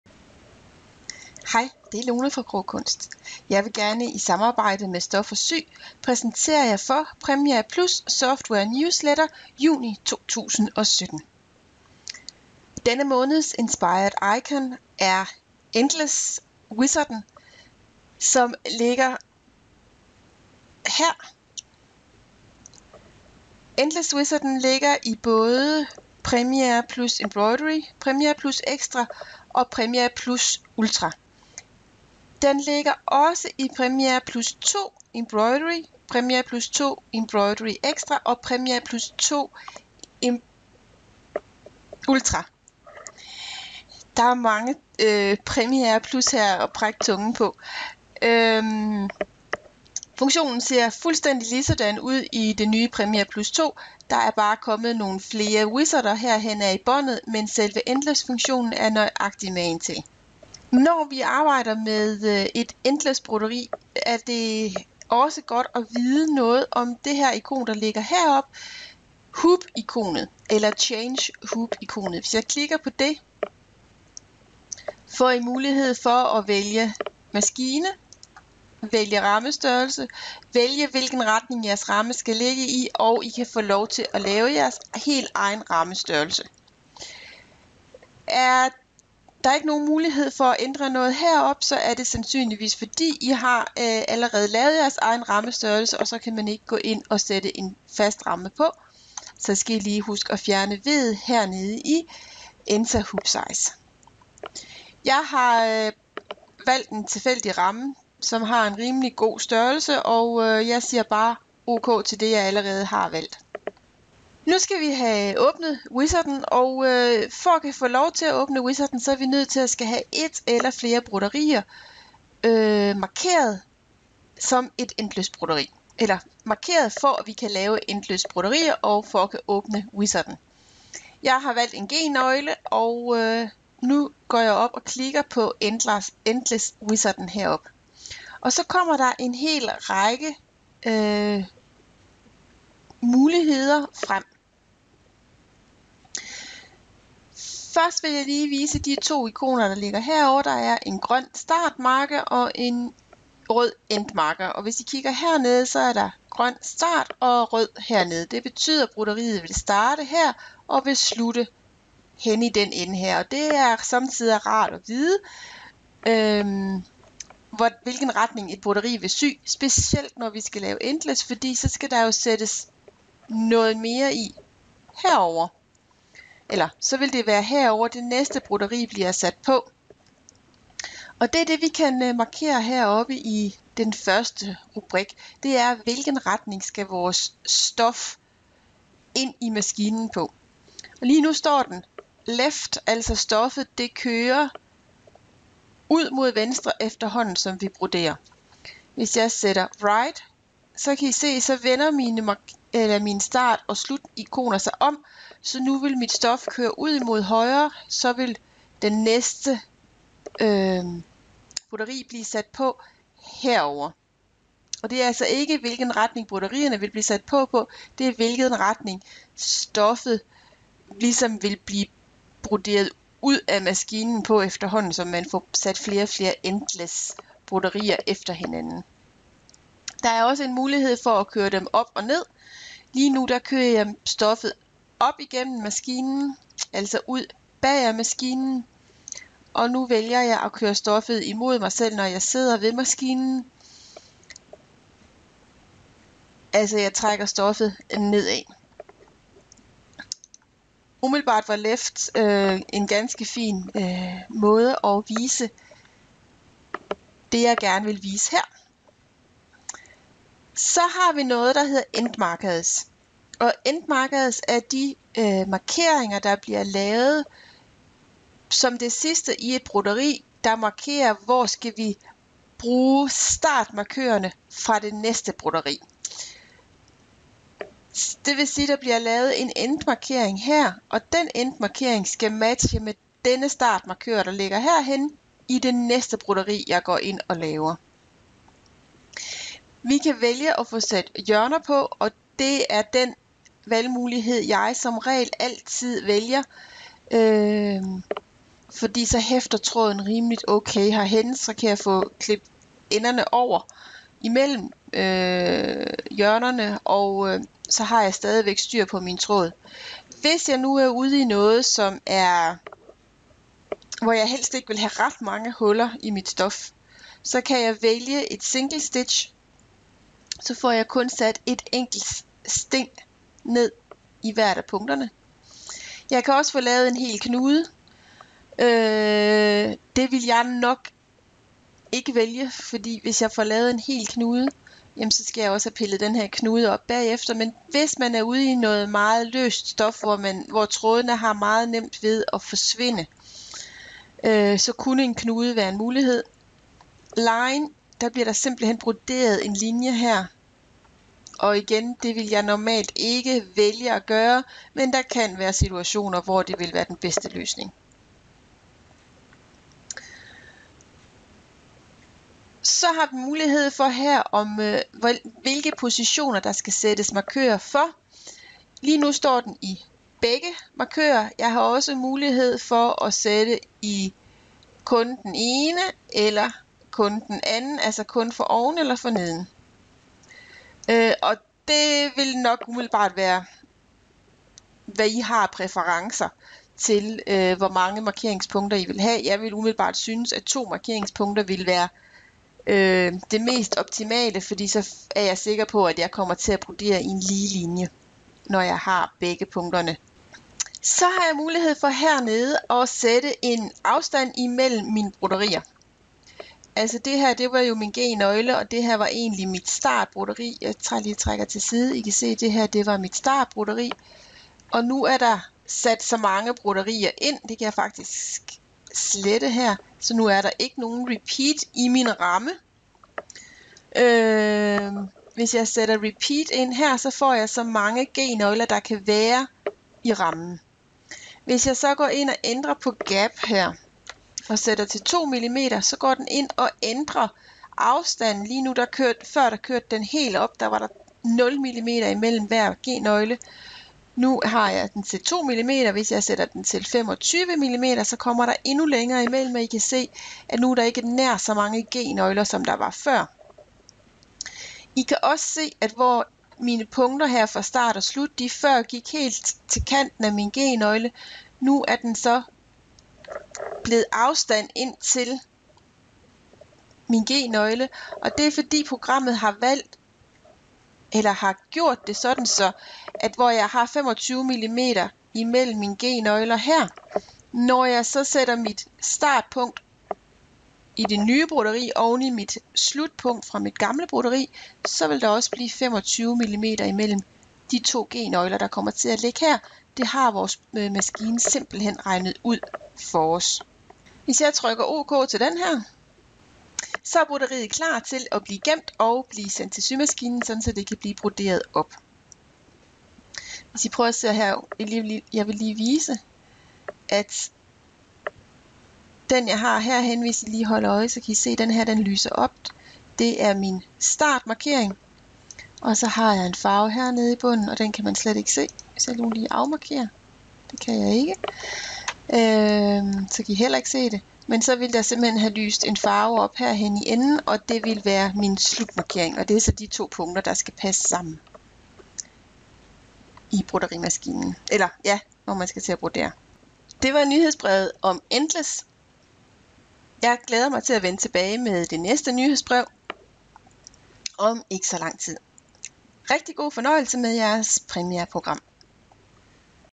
Premier nyhedsbrev juni 2017 - Endless
Nyhedsbrevet juni 2017 gennemgår Endless Wizarden og alle de smarte funktioner den har.
Nyhedsbrevet er frit oversat af Lone med tilladelse fra VSM. VSM har intet ansvar for Lones oversættelse.